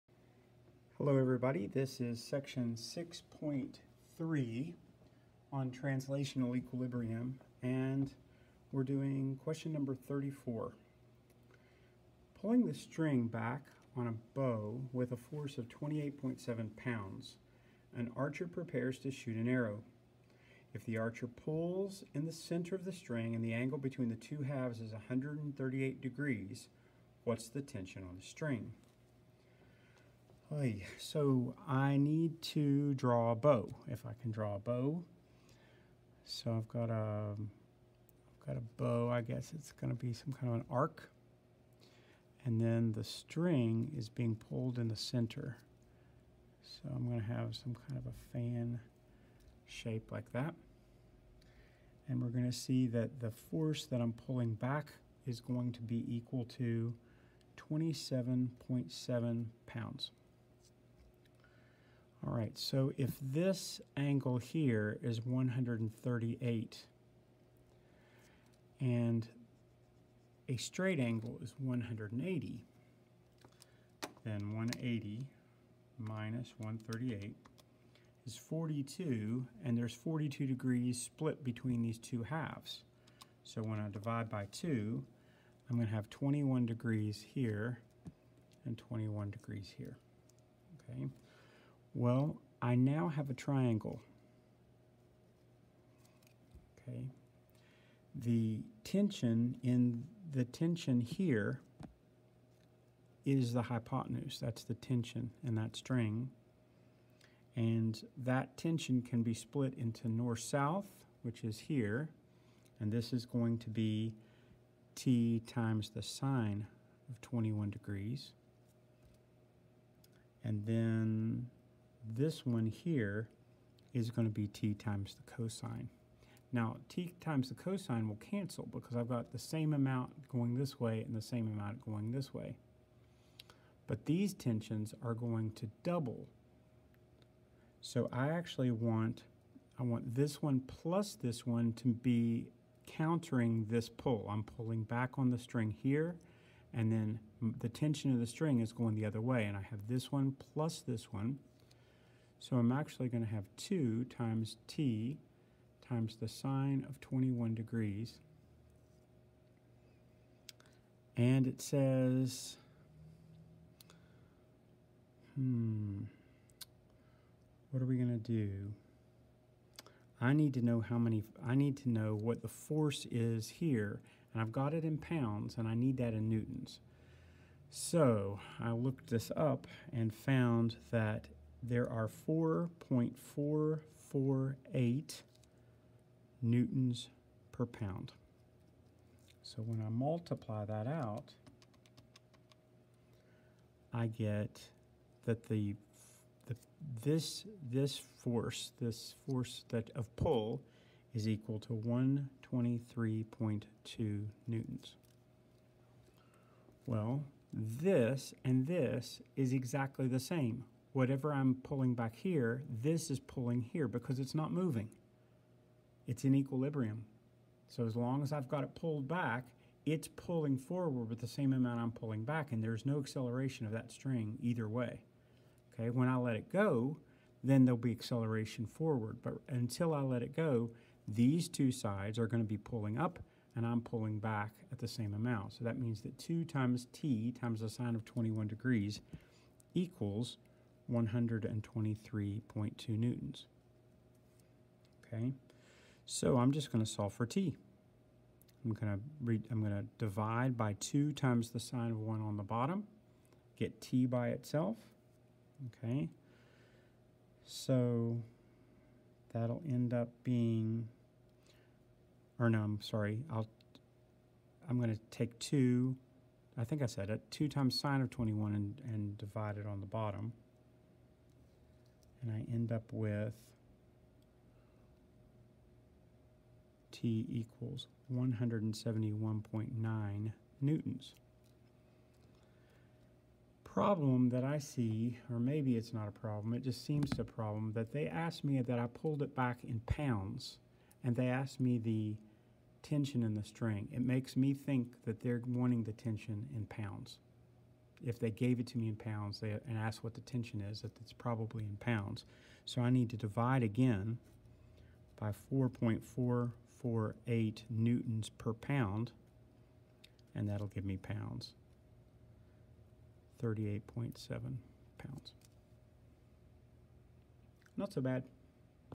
James Walker Physics 4th edition problem 6.34
Pulling the string on a bow back with a force of 28.7 lb, an archer prepares to shoot an arrow. If the archer pulls in the cen­ter of the string, and the angle between the two halves is 138°, what is the tension in the string?